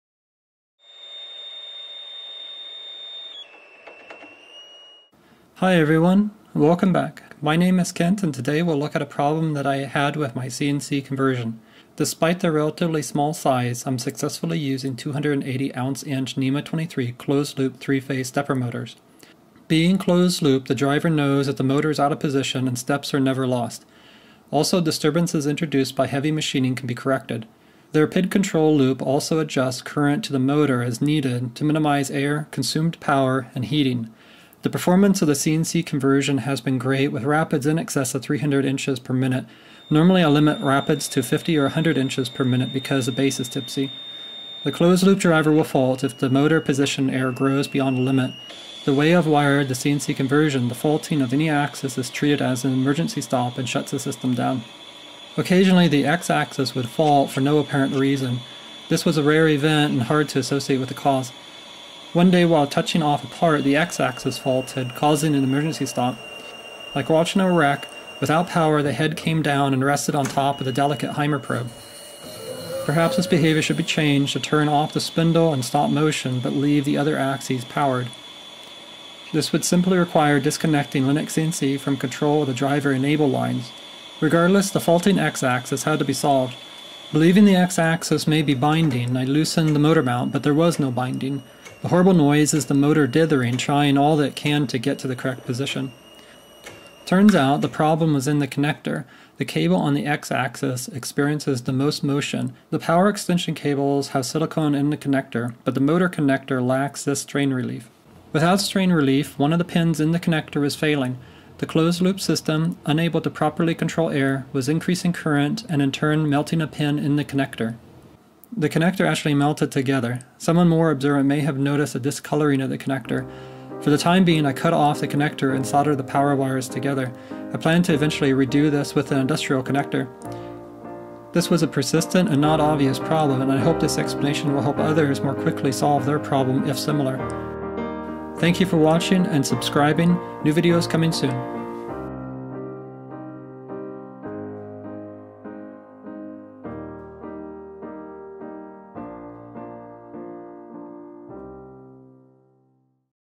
Mysteriously Faulting Stepper Motor, Closed-loop
The cause of an occasionally faulting closed-loop stepper motor is identified and repaired. I hope this short video will help others to avoid the same problem. The problem was inconsistent but after a Haimer problem was crushed, had to be solved. (All Haimer probes are doing well today.)